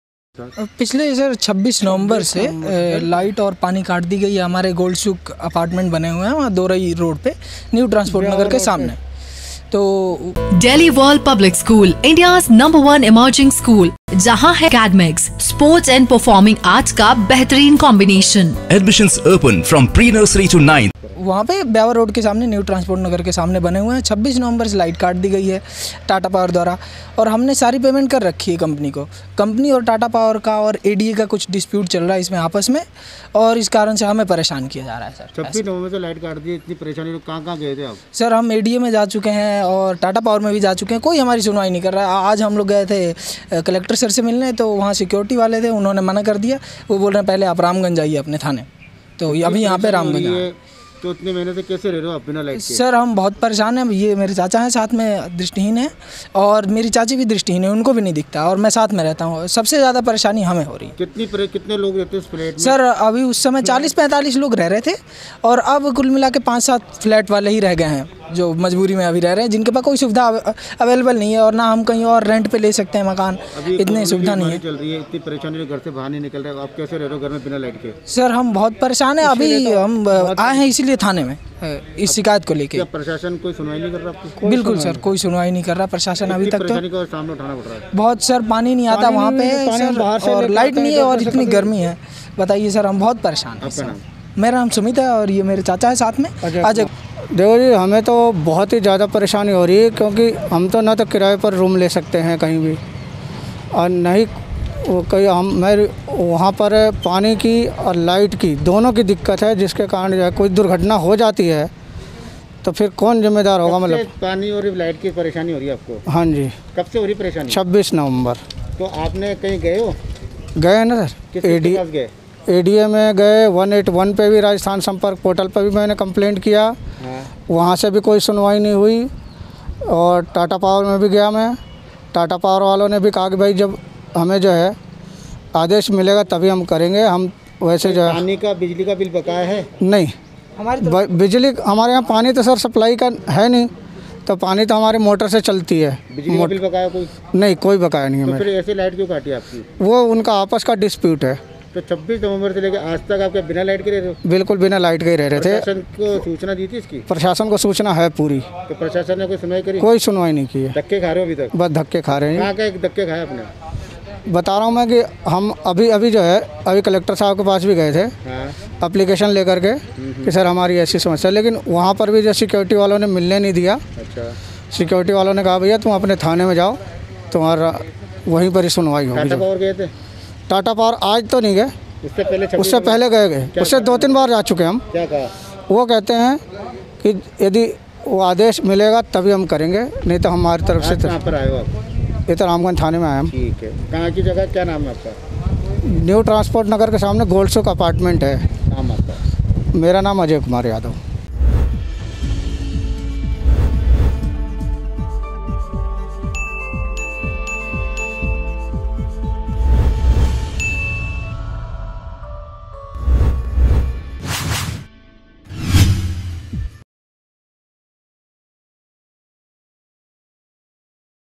AJMER NEWS | ट्रांसपोर्ट नगर में गोल्ड सुख अपार्टमेंट के लोग बिना बिजली रहने को मजबूर | MTTV INDIA
ट्रांसपोर्ट नगर में गोल्ड सुख अपार्टमेंट के लोग बिना बिजली रहने को मजबूर
26 नवंबर को टाटा पावर ने काटी बिजली
पीड़ित परिवार कलेक्टर के दर और टाटा पावर के सभी दफ्तरों के काट रहा है चक्कर
पीड़ित का आरोप एडीए और टाटा पावर का चल रहा है डिस्प्यूट जिसकी वजह से काटी गई बिजली